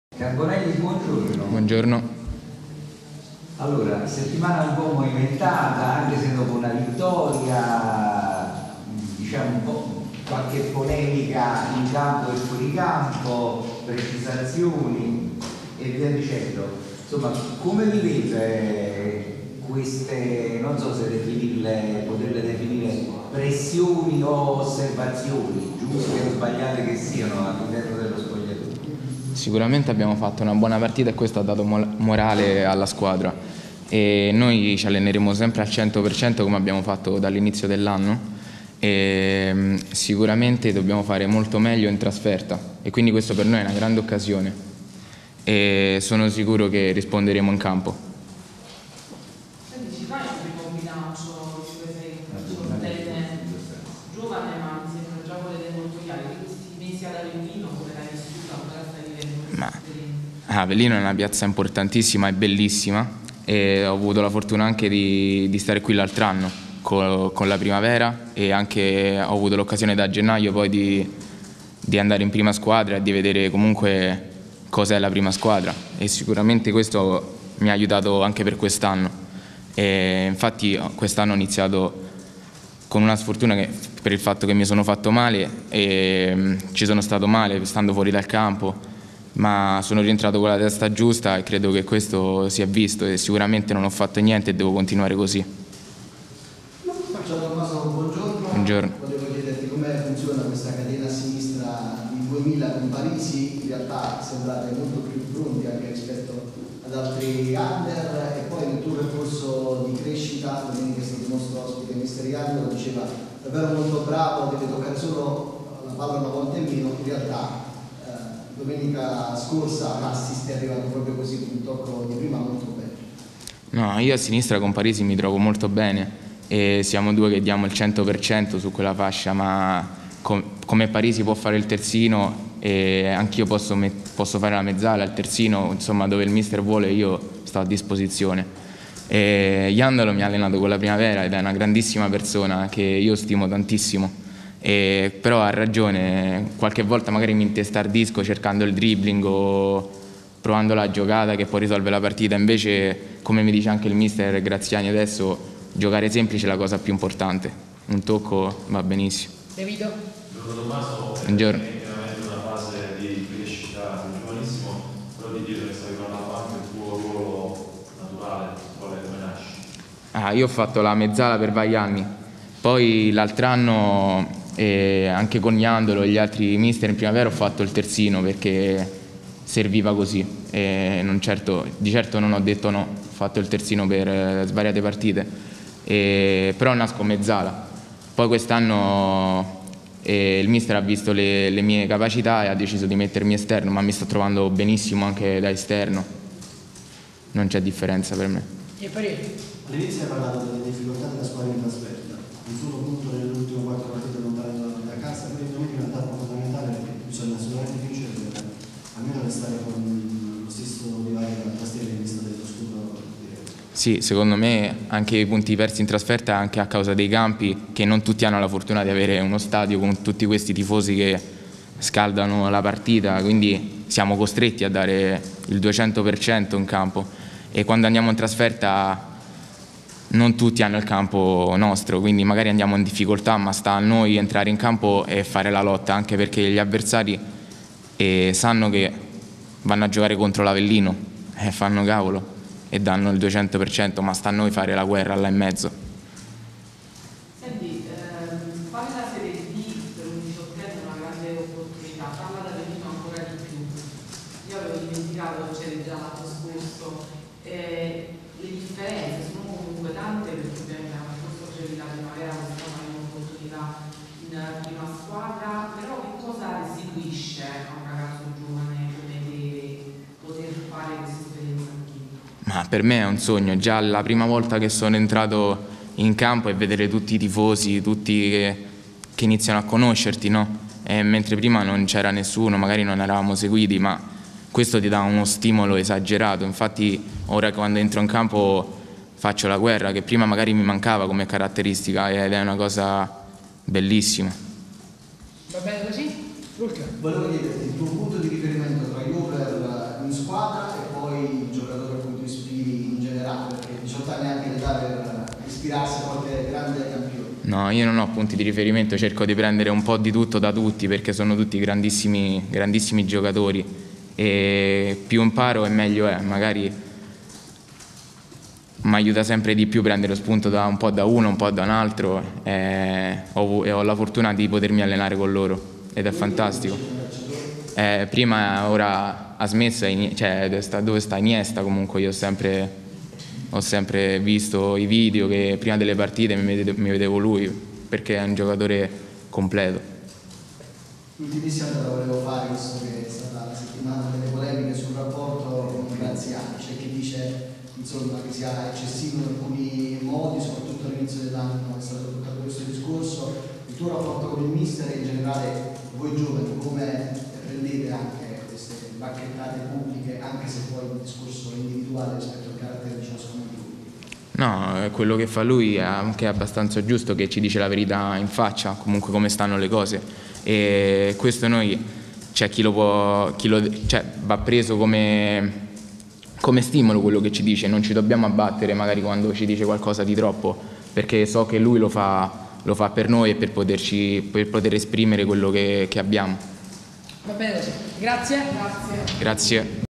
Verso Ostia Mare - Calcio Avellino, parla Carbonelli
Il centrocampista classe 2000 protagonista della consueta conferenza stampa infrasettimanale